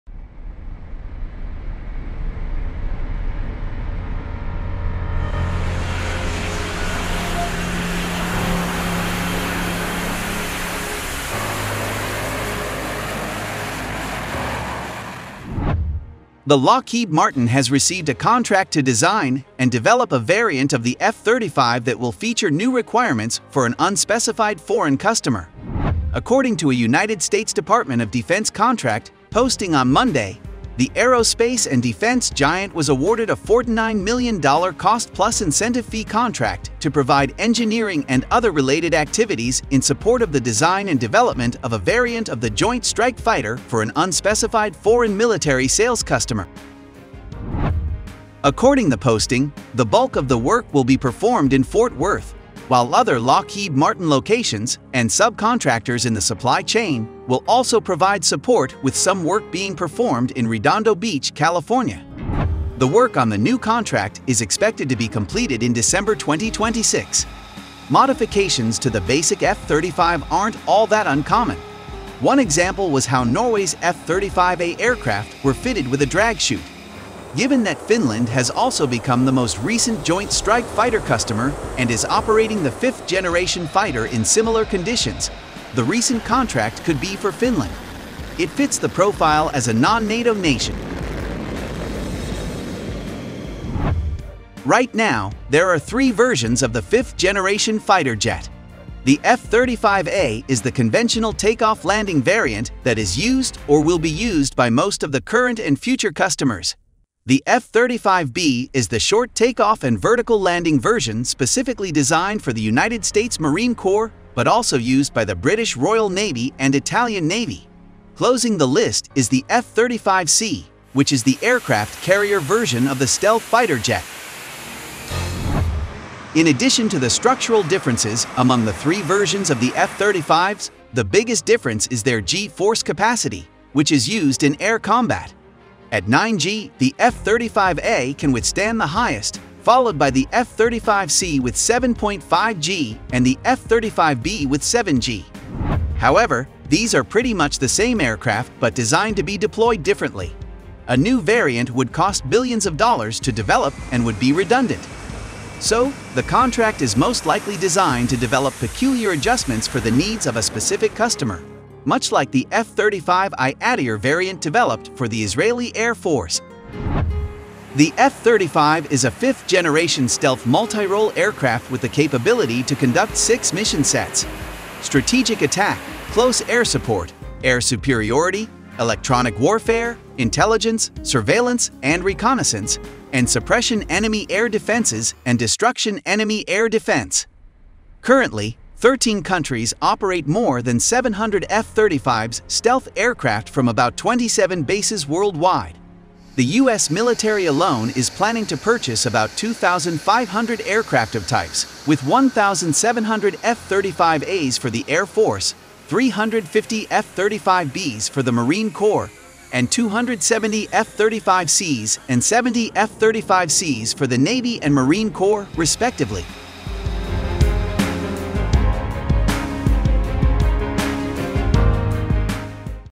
US Military Announced Their New Super F-35 Stealth Fighter
The F-35A is the conventional take-off landing variant that is used or will be used by most of the current and future customers. The F-35B is the short take-off and vertical landing (STOVL) version specifically designed for the United States Marine Corps but also used by the British Royal Navy and Italian Navy. Closing the list is the F-35C, which is the aircraft carrier version of the stealth fighter jet.

The F-35 is a 5th generation stealth multi-role aircraft with the capability to conduct six mission sets: Strategic Attack, Close Air Support, Air Superiority, Electronic Warfare, Intelligence, Surveillance, and Reconnaissance (ISR), and Suppression Enemy Air Defenses (SEAD) and Destruction Enemy Air Defense (DEAD).

Fifteen countries are operating or intend to operate the aircraft (the U.S., U.K., Australia, Canada, Japan, Israel, Italy, Norway, Denmark, Poland, Singapore, Belgium, South Korea, Netherlands, and Finland).